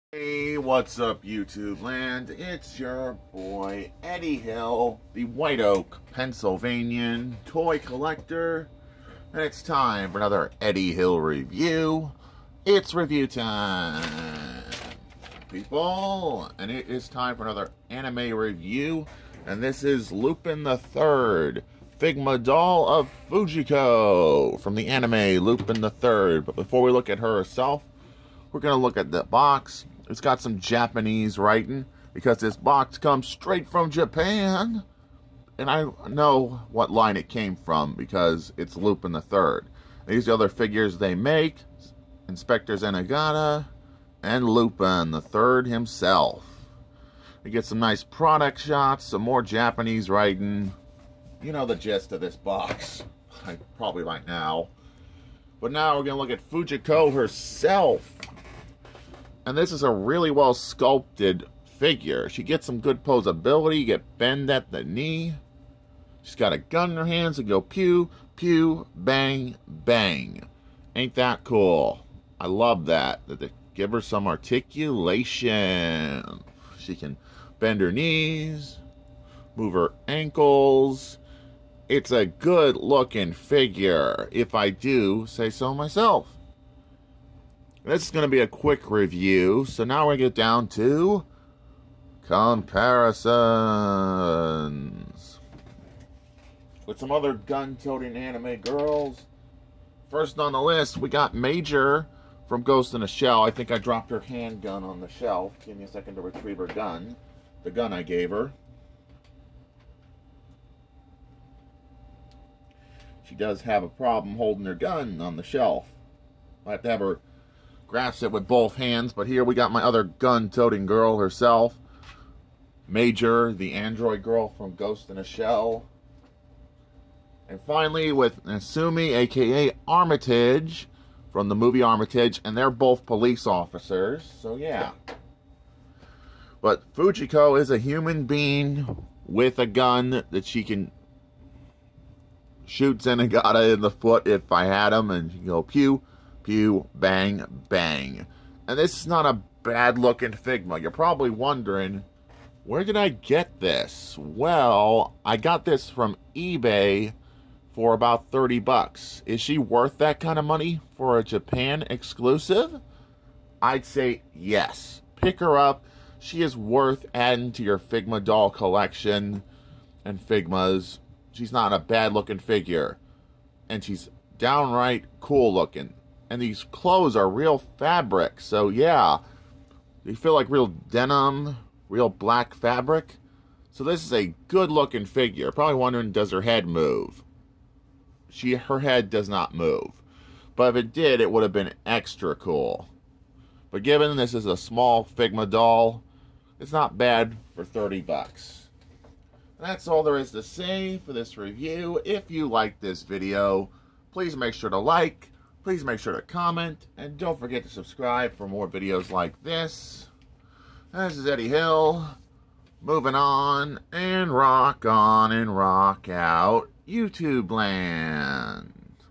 LUPIN THE THIRD FUJIKO FIGMA DOLL 1/15/23
EDDIE HILL REVIEWS: LUPIN THE THIRD FUJIKO FIGMA DOLL   1/15/23